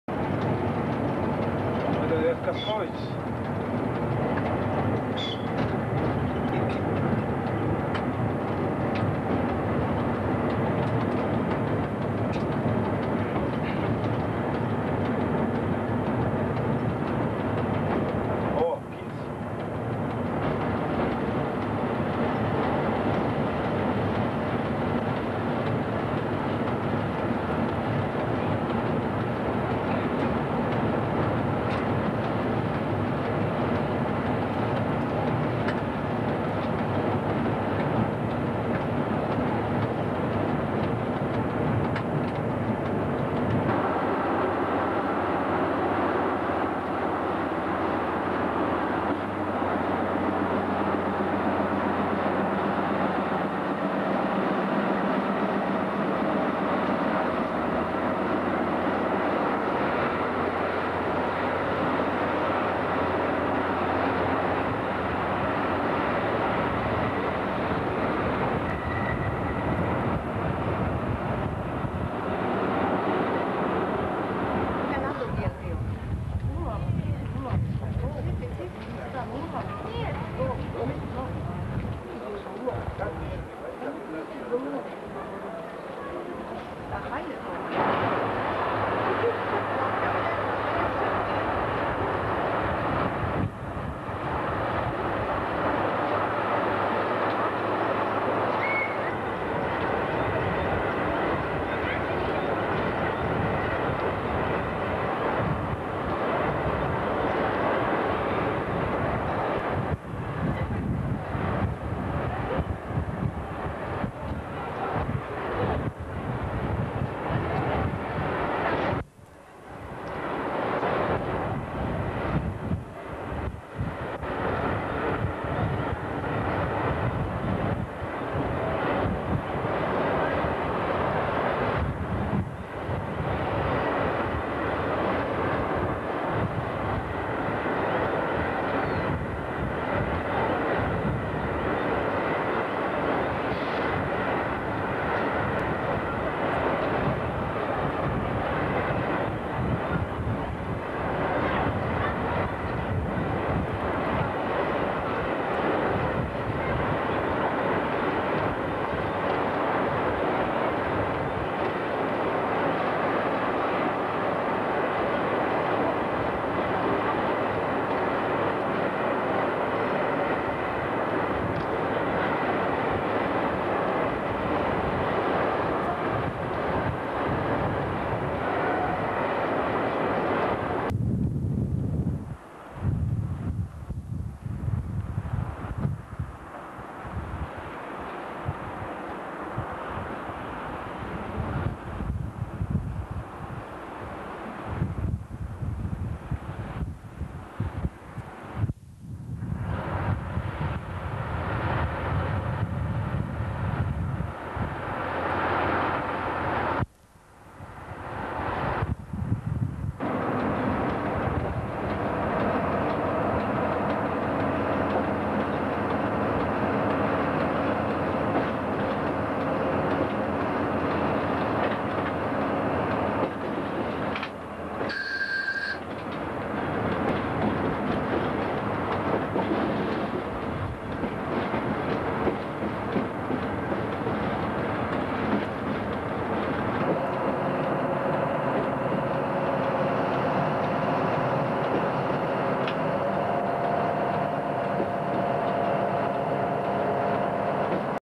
Станция Таллин-Балти в 27.04.1998 / Tallinn main station in 27.04.1998
Станция Таллин-Балти в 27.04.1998, Эстония
Автор: А. Роосма
Tallinn main station in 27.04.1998, Estonia
Author: A. Roosma